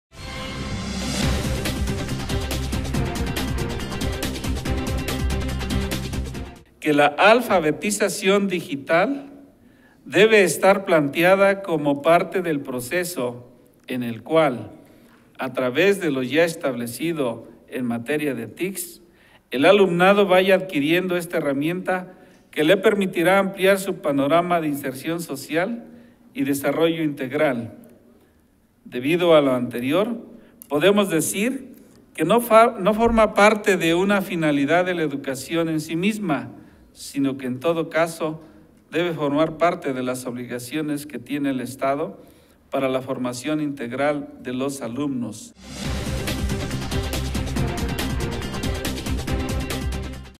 Eliseo Molina presentó iniciativa en materia de alfabetización digital
Molina Hernández consideró necesario reformar la fracción XV del artículo 14 de la Ley de Educación para el Estado de Hidalgo y establecer que la alfabetización digital debe estar planteada como parte del proceso en el cual, a través de lo ya establecido en materia de TIC´s, el alumnado vaya adquiriendo esta herramienta que le permitirá ampliar su panorama de inserción social, y desarrollo integral.